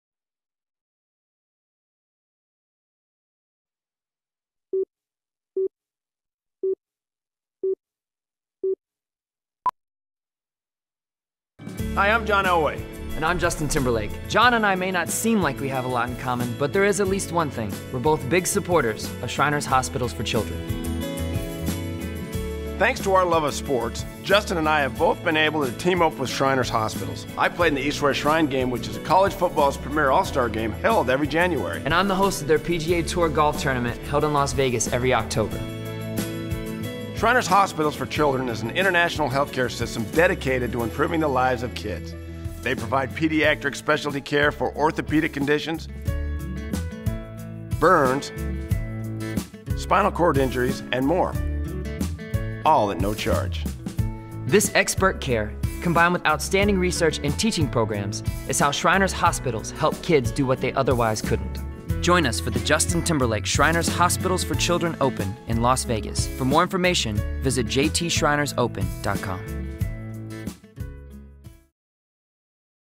Shriners Hospitals for Children Commerical
Commercial for Shriners Hospital with Justin Timberlake and John Elway